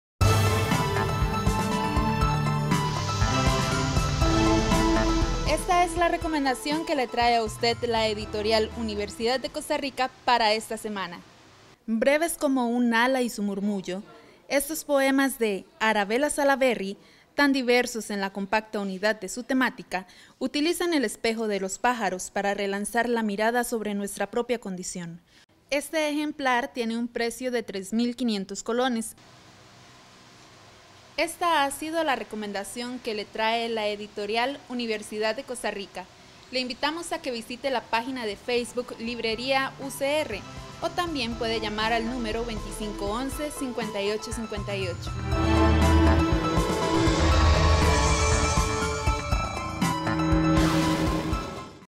Editorial 28 MAYO
La Editorial de la Universidad de Costa Rica nos recomienda el siguiente libro para esta semana.
Recuerde que puede participar en el sorteo de este libro, para hacerlo siga las instrucciones que se indican en nuestro perfil de Facebook.